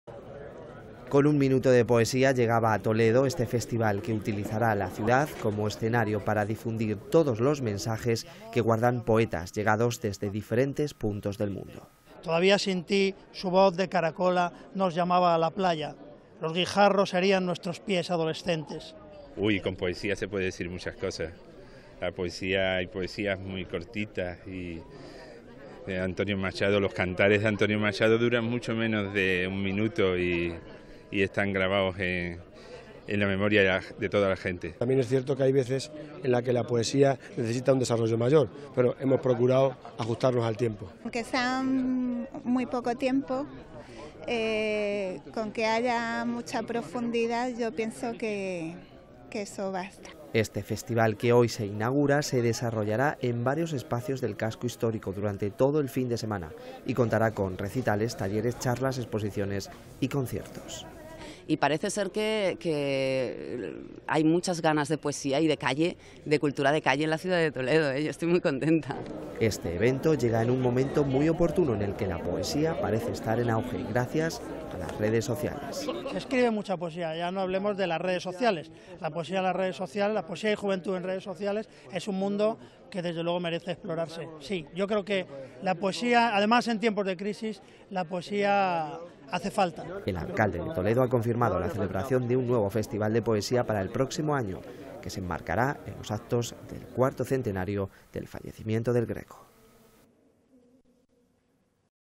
El festival de poesía llega a Toledo
El festival de poesía llega a Toledo, y lo hace recibiendo a poetas llegados desde todos los rincones del mundo. Durante todo el fin de semana se llevaran a cabo actividades, en diferentes espacios del Casco Histórico toledano.
Con un minuto de poesía llegaba a Toledo, este festival, que utilizara la ciudad como escenario para difundir todos los mensajes que guardan poetas llegados desde diferentes puntos del mundo.
El Festival VOIX Vives que hoy se ha inaugurado se desarrollará en varios espacios del Casco Histórico durante todo el fin de semana y contará con recitales, talleres, charlas, exposiciones, y conciertos.
El alcalde de Toledo ha confirmado la celebración de un nuevo festival de poesía, para el próximo año, y se enmarcaría en los actos del cuarto  centenario del fallecimiento del greco.